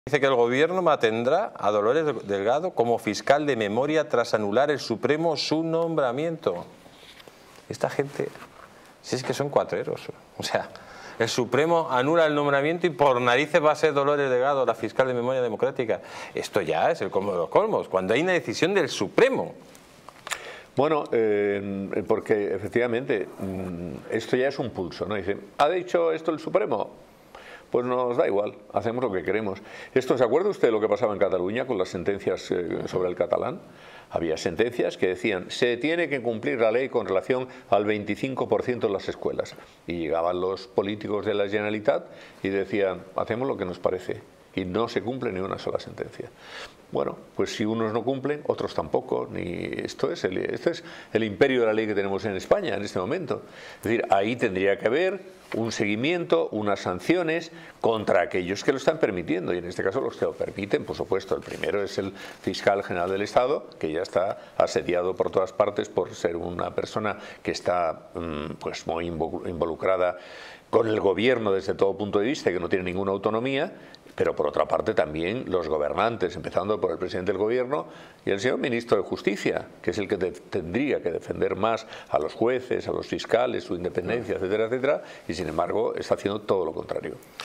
Gobierno mantendrá a Dolores Delgado como fiscal de «memoria» tras anular el Supremo su nombramiento
El Gobierno mantendrá a Dolores Delgado como fiscal de "memoria" tras anular el Supremo su nombramiento. ¡Más detalles en Distrito TV!